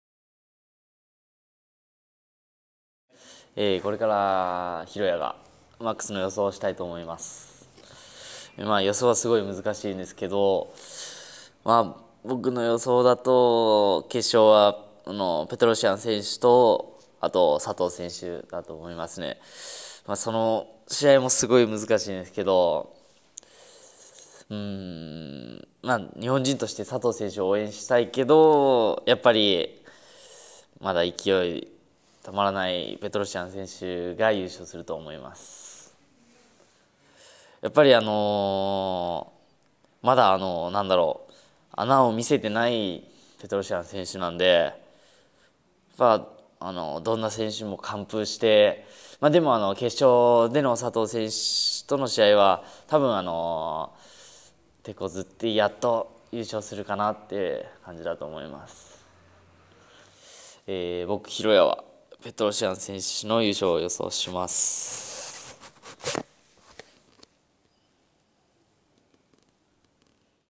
HIROYA's Picks!! - 2010 MAX FINAL
HIROYA says he knows who will win the MAX Final!? Special Predictions for FieLDS K-1 WORLD MAX 2010 -70kg World Championship Tournament FINAL (2010 Nov,8 / Ryogoku Kokugikan, Tokyo, JAPAN).

HIROYAがMAXトーナメントの勝ち上がりを大予想！　11・8『FieLDS K-1 WORLD MAX 2010 -70kg World Championship Tournament FINAL』両国大会の行方はどうなる!?